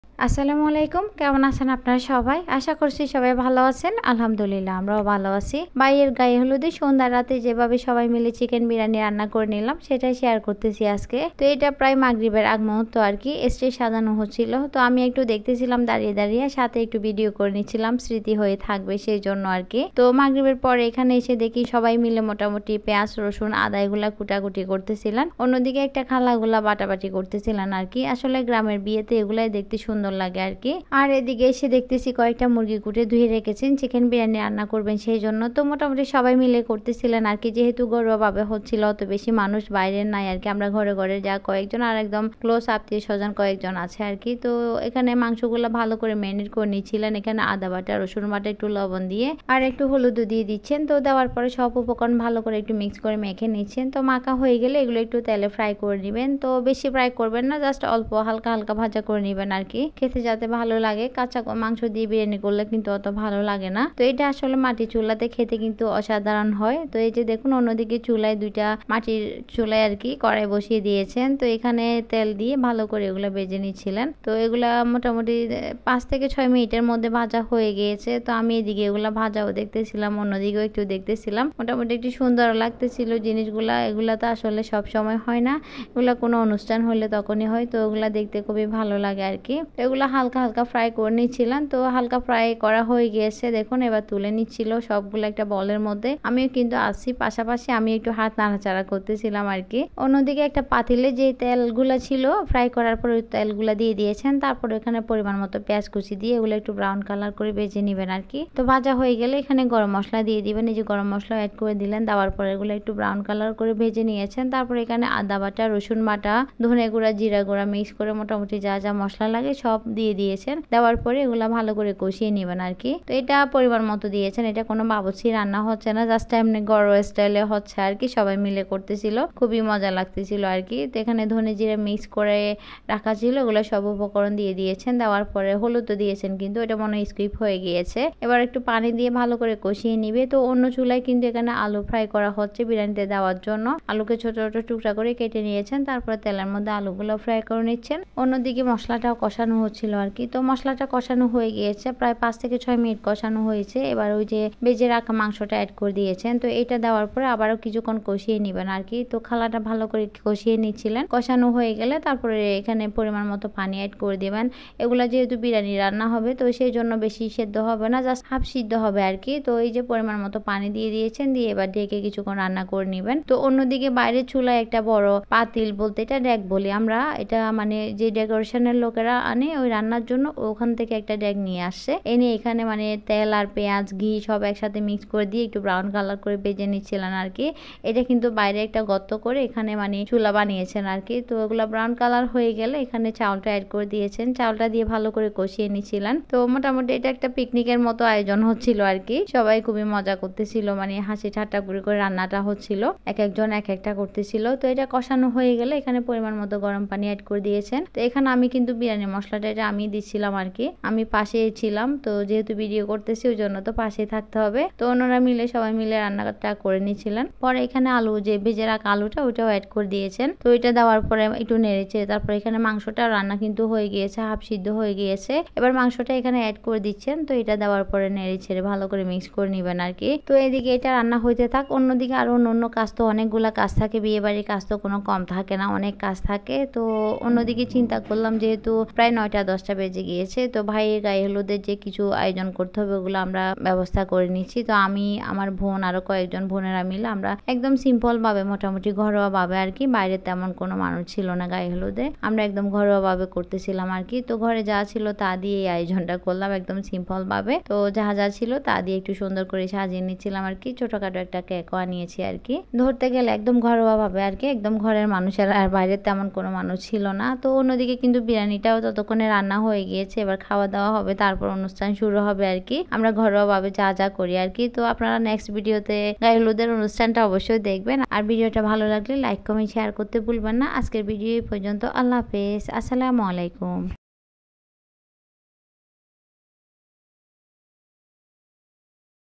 ভাইয়ের গায়ে হলুদে যে ভাবে সবাই মিলে চিকেন বিরিয়ানি রান্না করে নিলাম ||Alo's recipe & vlog।
@Alo'srecipe&vlog.